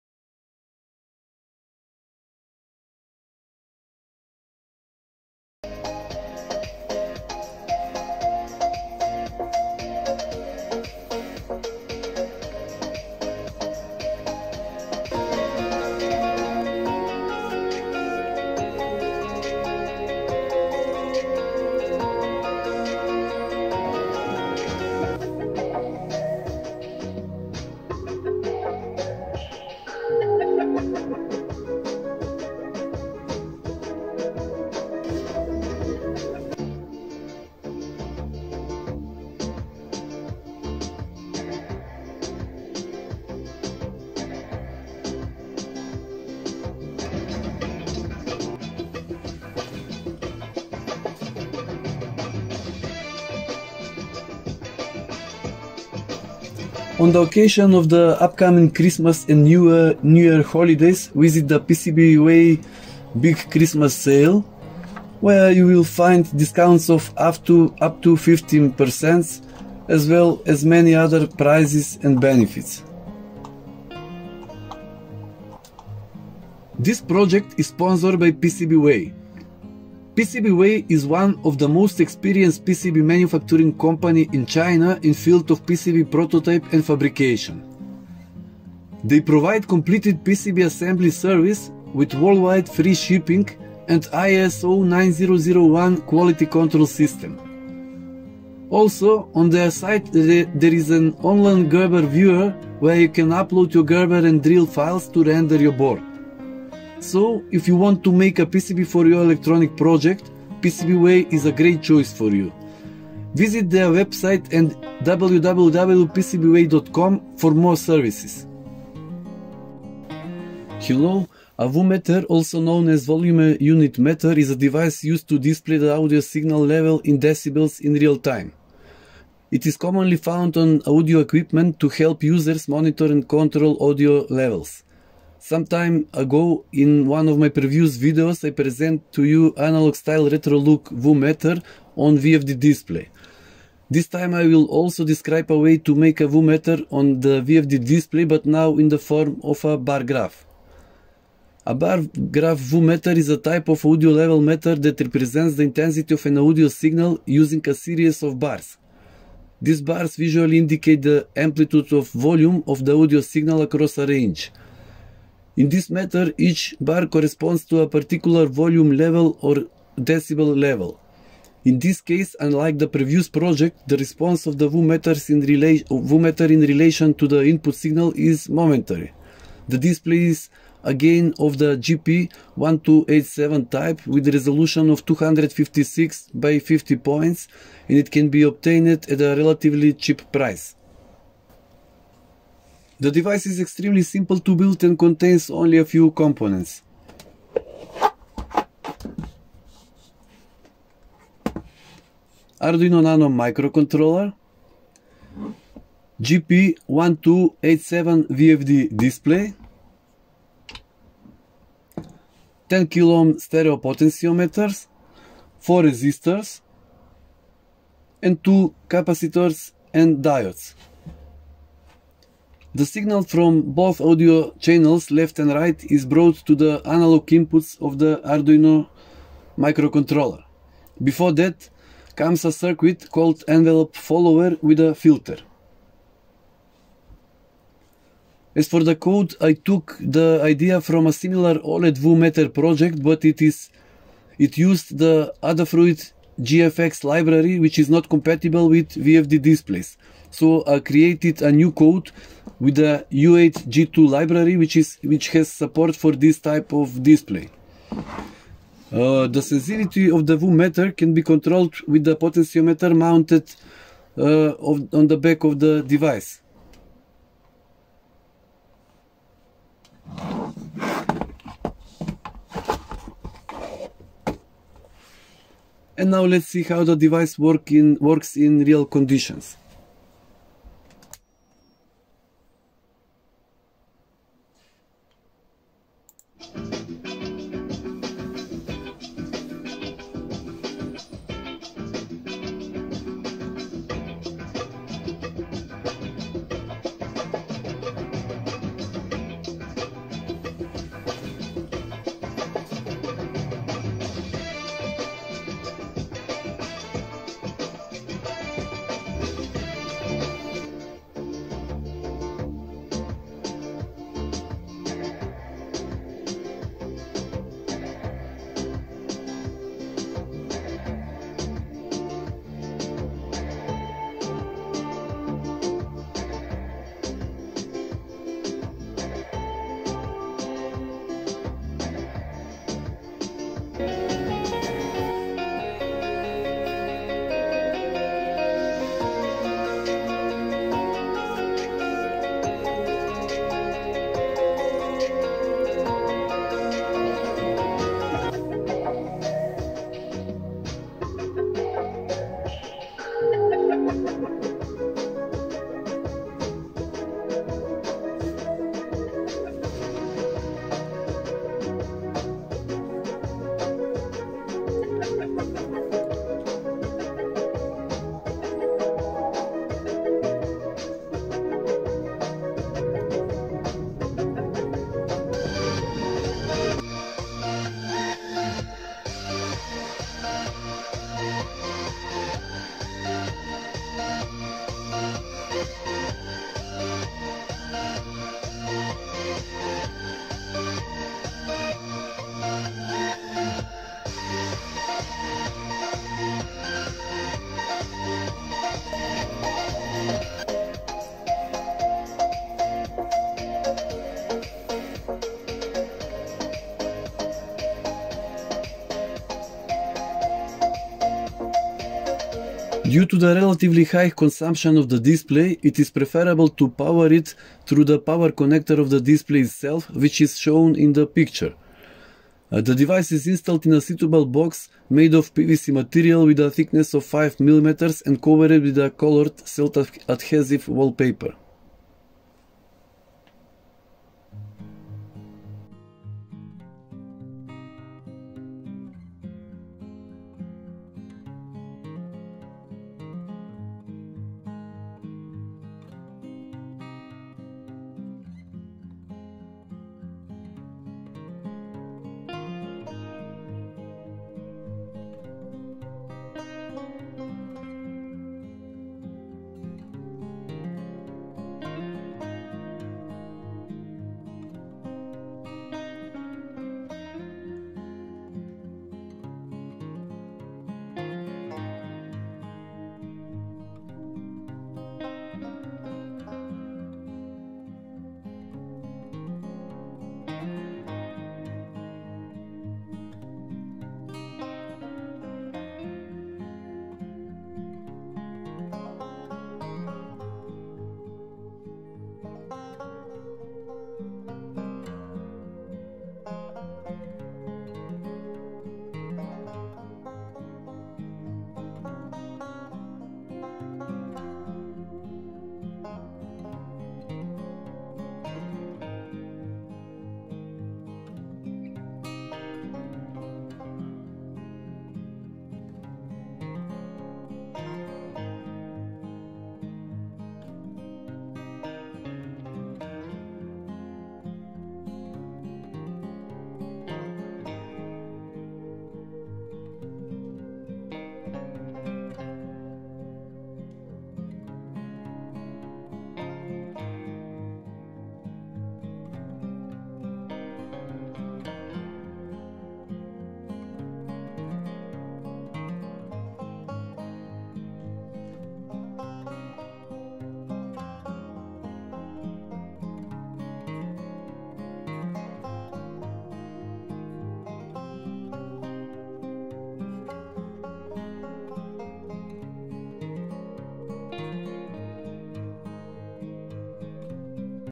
Arduino Bar graph Stereo VU meter on VFD display GP1287
A bar graph VU (Volume Unit) meter is a type of audio level meter that represents the intensity of an audio signal using a series of bars. These bars visually indicate the amplitude or volume of the audio signal across a range.
    The device presented in this video is extremely simple to build and contains only a few components
  - Arduino Nano Microcontroller
  - GP1287 VFD Display
  - 10K stereo potentiometer,
  - Four resistors,
  - and two capacitors and diodes
The signal from both audio channels (left and right) is brought to a circuit called "enveope follower", and then to the analog inputs of the Arduino microcontroller. I created a code with the U8G2 library, which has support for this type of display.

Chapters:
0:06 Short Demo
2:02 Description
3:13 Inside
4:40 Demonstration
6:52 Power Consumption
7:16 Building